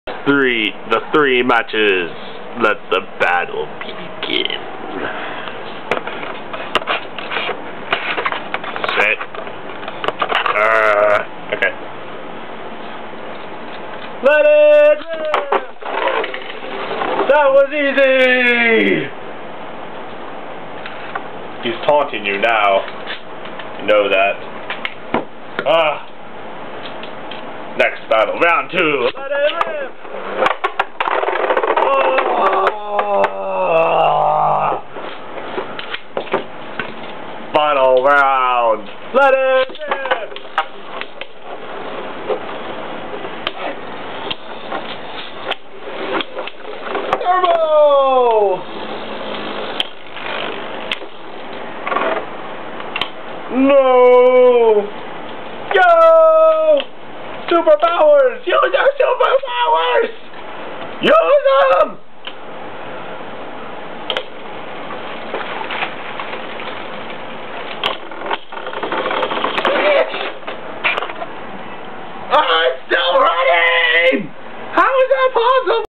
Beyblade Battle 1
Loud Announcer guy faces against the tough to beat Silent Guy!